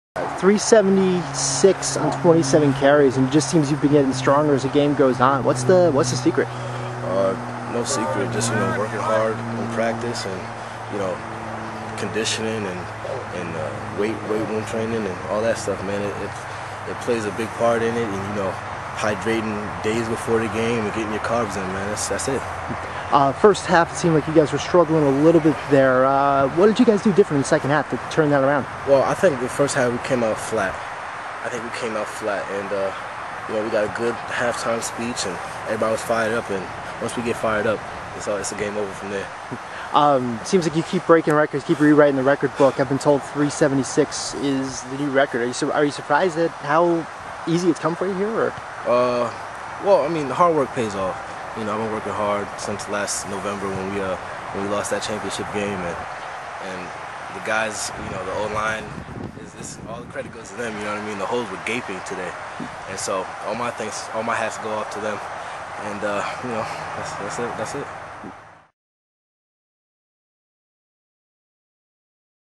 Silas Redd Postgame Interview
King School running back Silas Redd talks with Tim Parry of the FCIAC Football Blog after Penn State-bound Redd rushed for 356 yards and 6 touchdowns against Hamden Hall on Sept. 27, 2009.